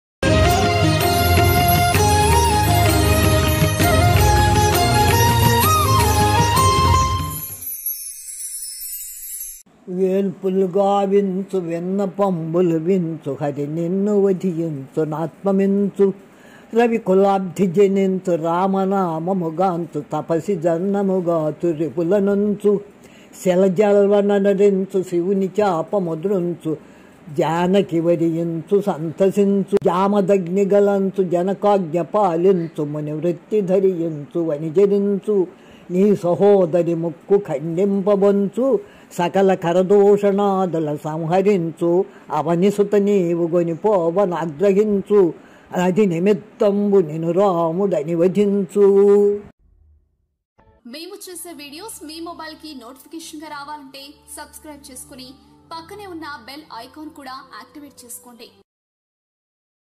రావణ వధ
రావణుని వధ ఒకే ఒక్క పద్యంలో .